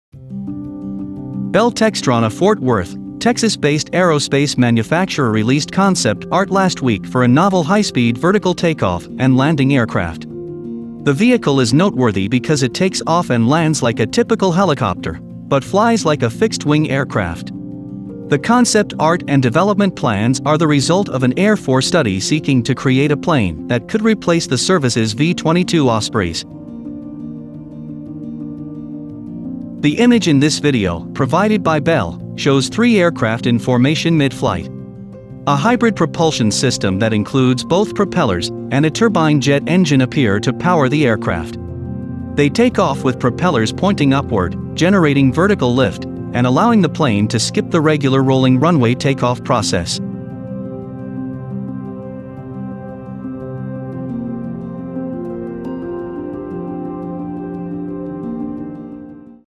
This Hybrid Aircraft Could Replace the V-22 Osprey
Bell Textron Inc., a Textron Inc. company, announced today the unveiling of design concepts for new aircraft systems for military applications which would use Bell's High-Speed Vertical Take-Off and Landing (HSVTOL) technology as the company continues its innovation of next generation vertical lift aircraft. HSVTOL technology blends the hover capability of a helicopter with the speed, range and survivability features of a fighter aircraft.

“Bell’s HSVTOL technology is a step change improvement in rotorcraft capabilities,” said Jason Hurst, vice president, Innovation. “Our technology investments have reduced risk and prepared us for rapid development of HSVTOL in a digital engineering environment, leveraging experience from a robust past of technology exploration and close partnerships with the Department of Defense and Research Laboratories.”